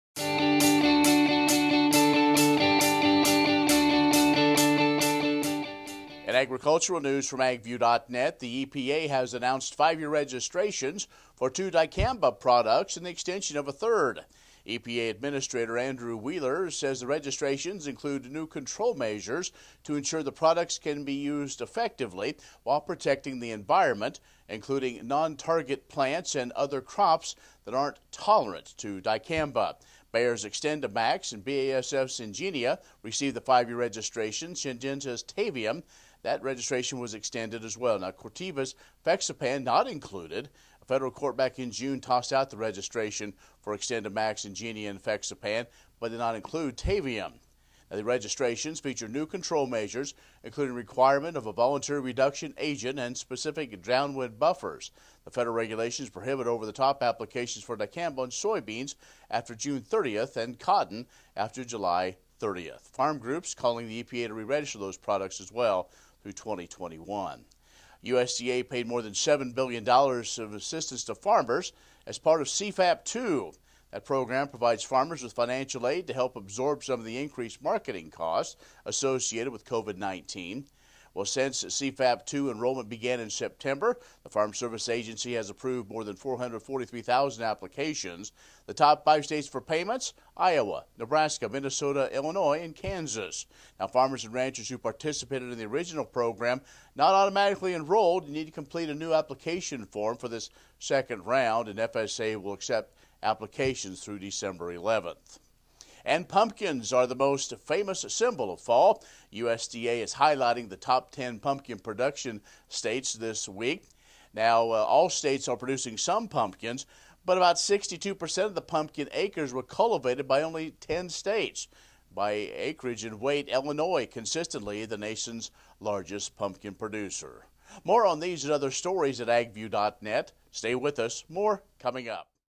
Authentic Ag News November 6, 2020
Ken Rahjes: In agricultural news from agview.net, the EPA has announced five-year registrations for two dicamba products and the extension of a third. EPA administrator, Andrew Wheeler, says the registrations included new control measures to ensure the products can be used effectively, while protecting the environment, including non-target plants and other crops that aren't tolerant to dicamba. Bayer's XtendiMax and BASF's Engenia received the five-year registration. Syngenta's Tavium, that registration was extended as well. Now, Corteva's FeXapan not included. A federal court back in June tossed out the registration for XtendiMax, Engenia and FeXapan, but did not include Tavium.
The registrations feature new control measures, including requirement of a voluntary reduction agent and specific downwind buffers. The federal regulations prohibit over-the-top applications for dicamba on soybeans after June 30th, and cotton after July 30th. Farm groups are calling the EPA to re-register those products as well through 2021.
USDA paid more than $7 billion of assistance to farmers as part of CFAP 2. That program provides farmers with financial aid to help absorb some of the increased marketing costs associated with COVID-19. Since CFAP 2 enrollment began in September, the Farm Service Agency has approved more than 443,000 applications. The top five states for payments, Iowa, Nebraska, Minnesota, Illinois and Kansas. Now, farmers and ranchers who participated in the original program, not automatically enrolled. You need to complete a new application form for this second round. FSA will accept applications through December 11th.
Pumpkins are the most famous symbol of fall. USDA is highlighting the top 10 pumpkin production states this week. Now, all states are producing some pumpkins, but about 62% of the pumpkin acres were cultivated by only 10 states. By acreage and weight, Illinois is consistently the nation's largest pumpkin producer. More on these and other stories at agview.net. Stay with us, more coming up.